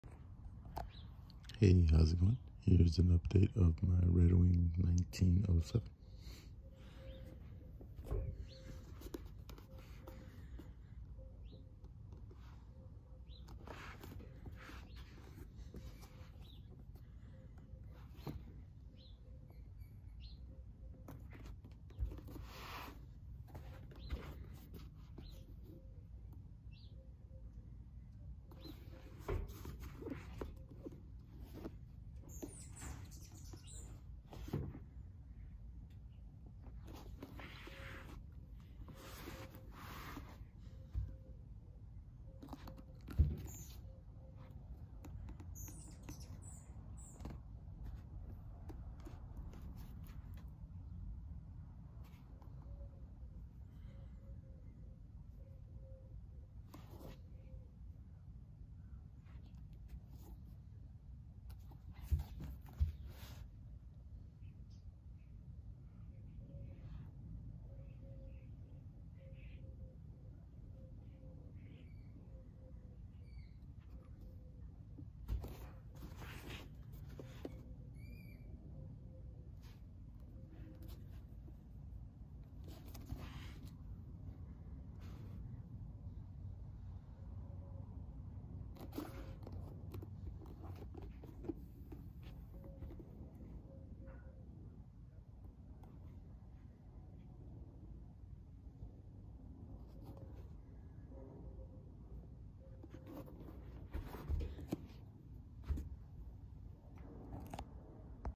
Red Wing 1907 6” Moc Toe - 7 Year Update
These need a resolve, only really use them in the fall now a days